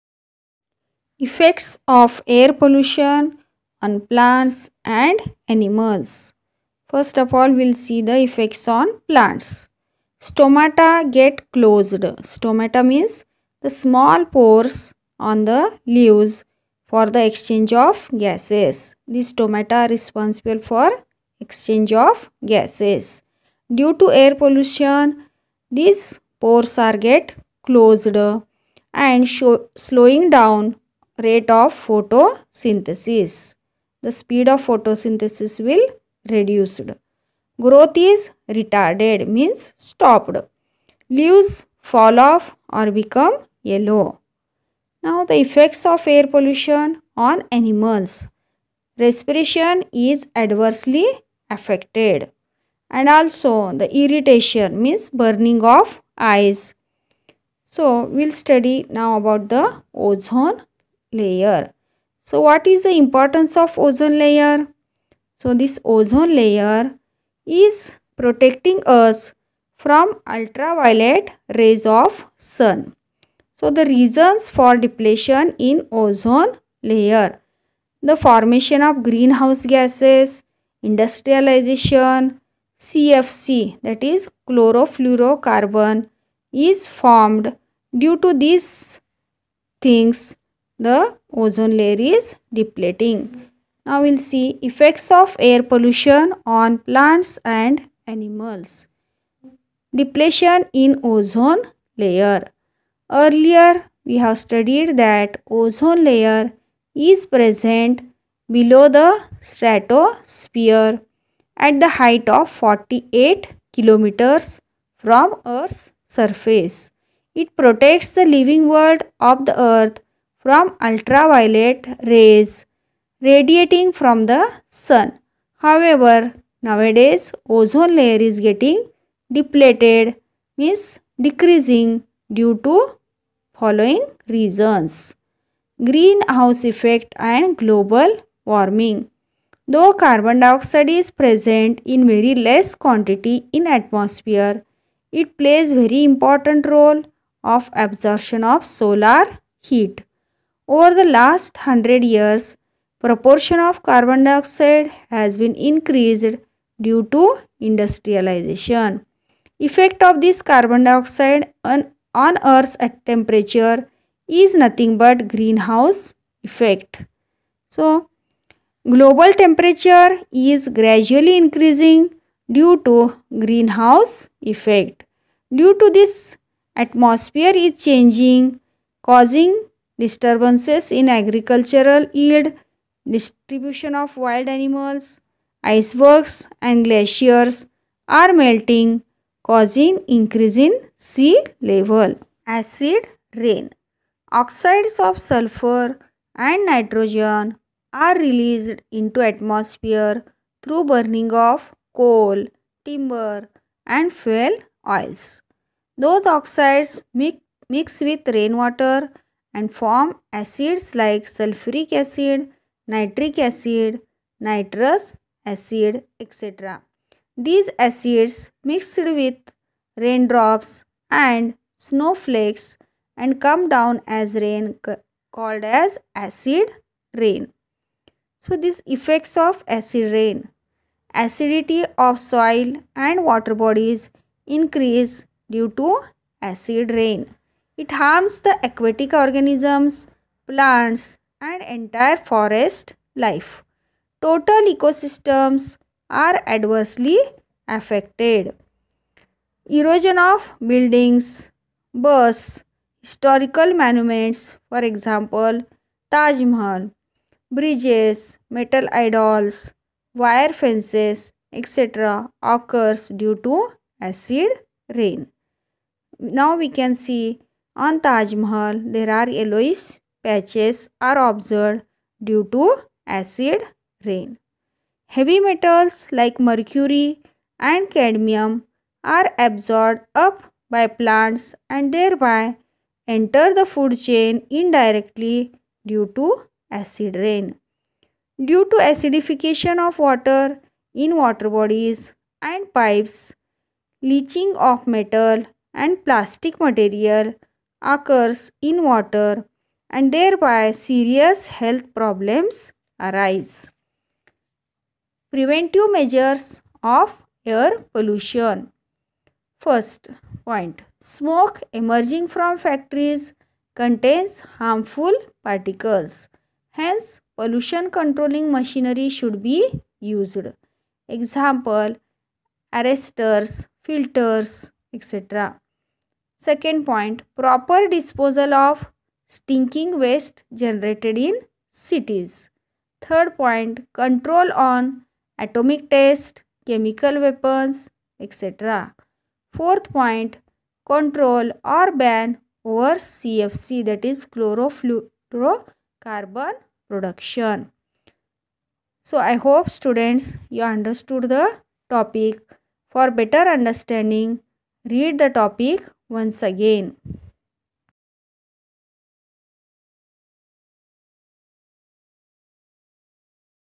std 8th Lesson 8. Pollution |part 2 🏭🌫️
std 8th
Lesson 8. Pollution part 2
Effects of air pollution
By Kulkarni Tr.